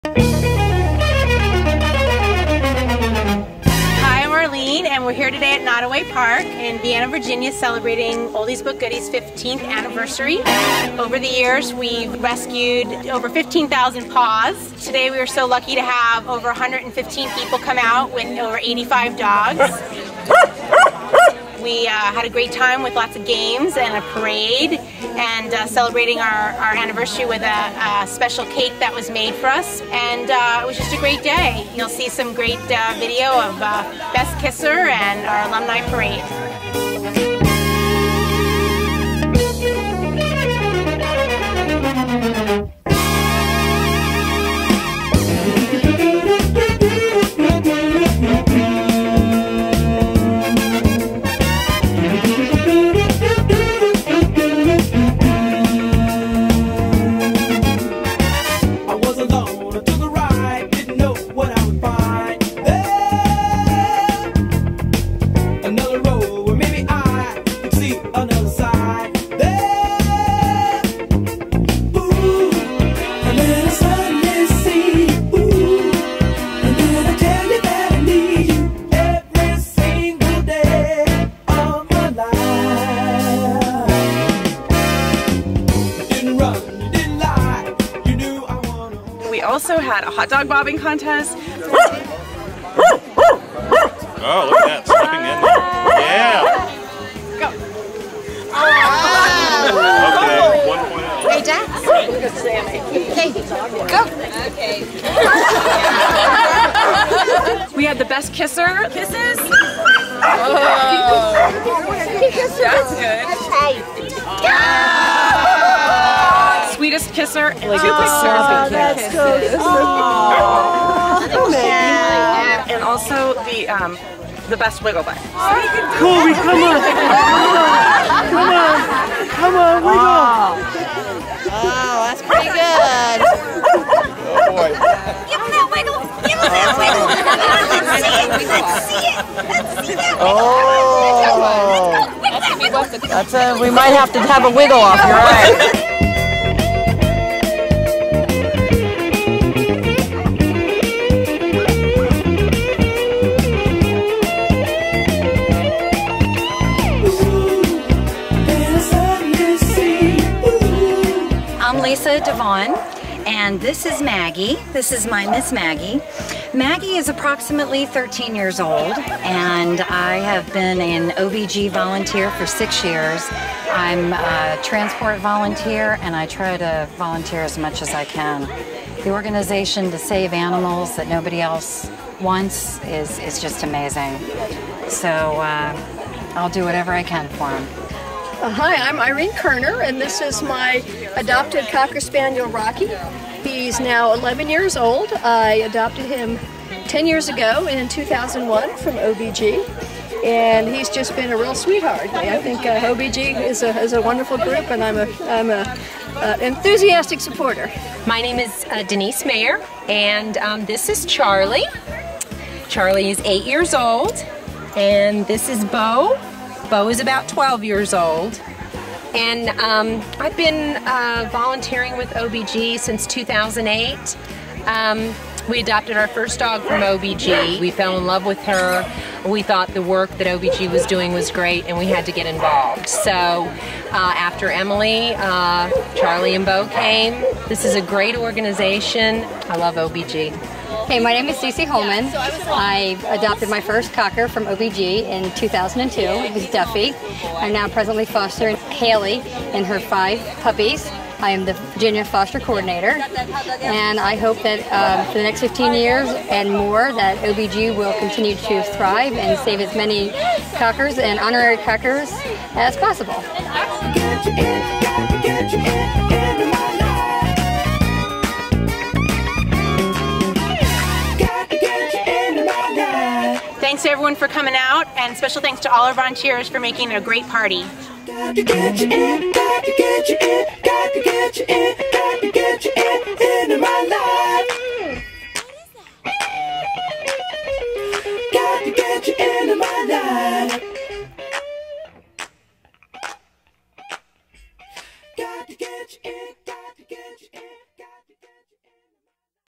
OBG's 15th Anniversary Party
Please enjoy this inside look at Oldies But Goodies Cocker Spaniel Rescue's 15th Anniversary Party at Nottoway Park in Vienna, VA. We had a great turn out of people and wigglebutts.

For more information on OBG and on the dogs available for adoption please go

We will also be posting a "kiss cam" from the party soon!